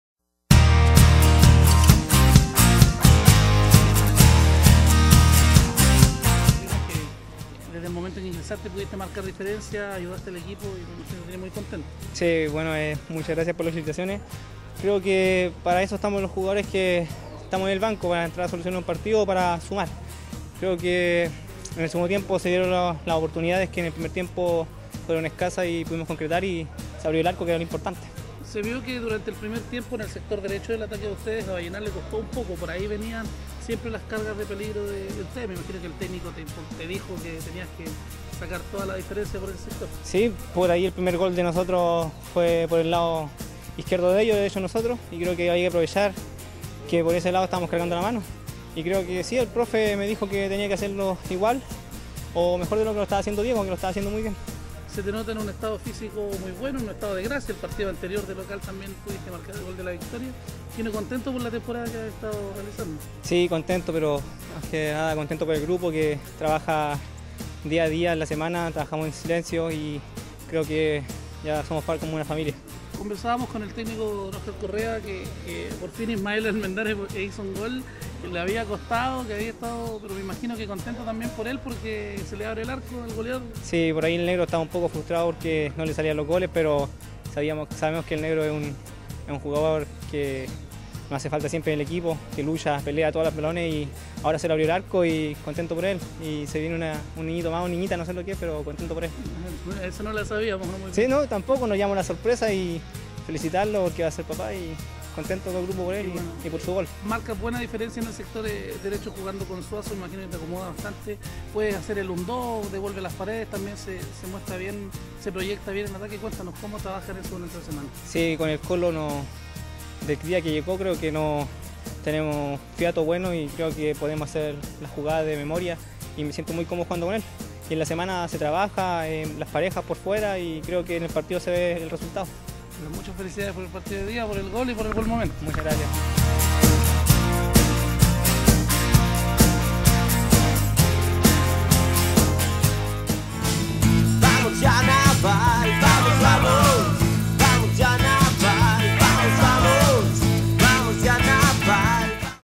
Fabian Alarcon se refiere al triunfo del fin de semana ante Vallenar
El lateral navalino Fabián Alarcón ingresó en el complemento del partido ante Deportes vallenar y fue el elemento clave para la victoria. Sus declaraciones en exclusiva para Naval TV.

Dale "me gusta" a los videos y difunde el rico historial del club.